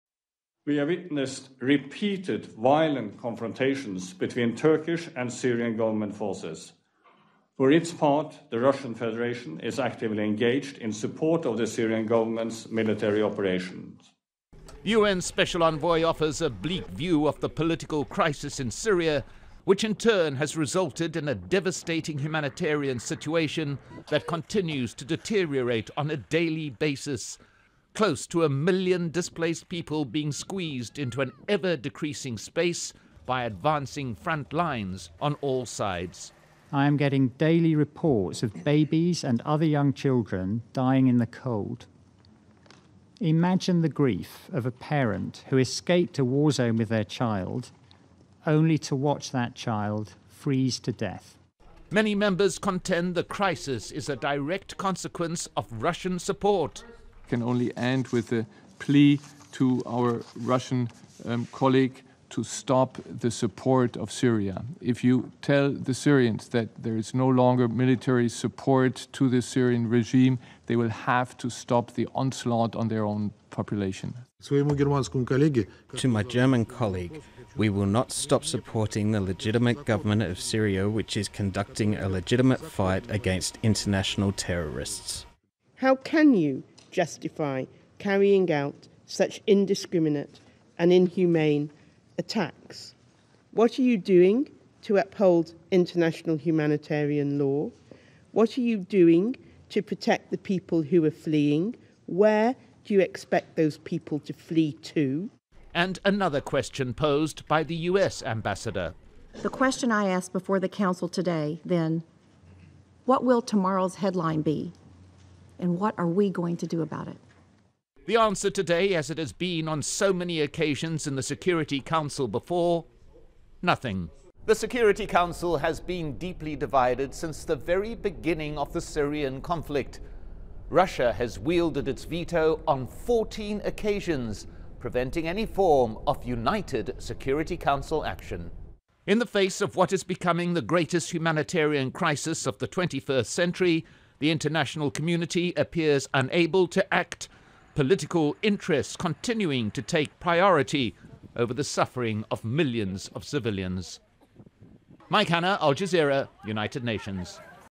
'Nowhere is safe': UN warns of urgent danger of Syria escalation
The United Nations has warned of "catastrophic human suffering" in Syria as hundreds of thousands flee to northwards near Turkey's border.
The Syrian government backed by Russian air power continue to attack the last rebel stronghold in the northwest.
World powers at a meeting at the UN to stop the onslaught warned that babies had frozen to death.
Al Jazeera's Mike Hanna reports from the UN in New York.